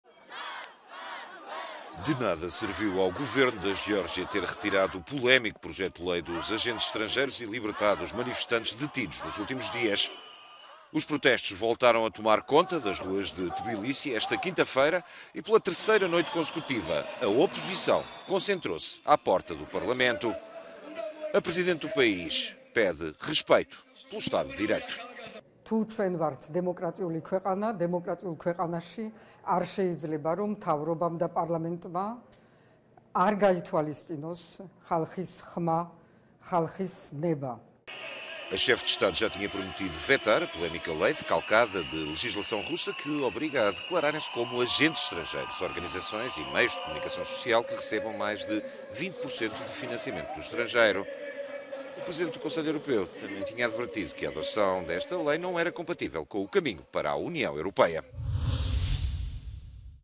Fim da lei dos "agentes estrangeiros" não trava protestos na Geórgia
Protestos não param. De nada serviu ao governo da Geórgia ter retirado o polémico projeto de lei dos "agentes estrangeiros" e libertado os manifestantes detidos nos últimos dias.